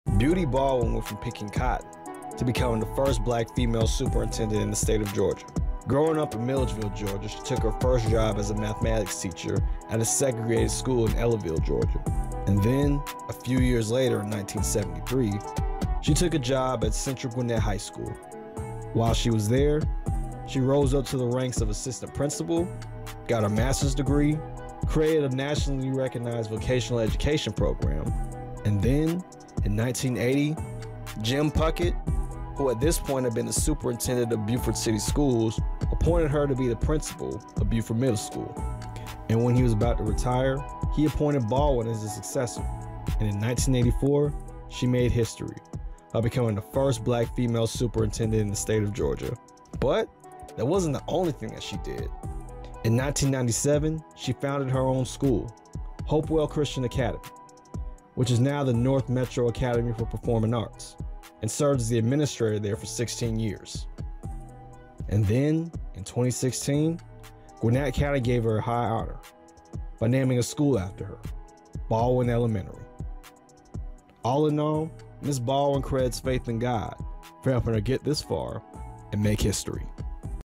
They Named a School after Her: The Beauty Baldwin Story
Beauty Baldwin made history in the 80s by becoming the first Black Woman Superintendent in the State of Georgia.  Almost 30 years later, Gwinett County named a school after her.